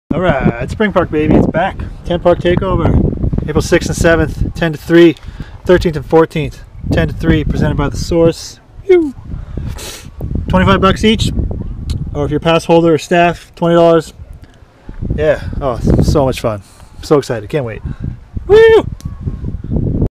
Temp Park Takeover 2019
Spring park at Rabbit Hill dual bunny ropes
April 6th&7th            13th&14th Presented by The Source
$25 per or $20 pass holders and staff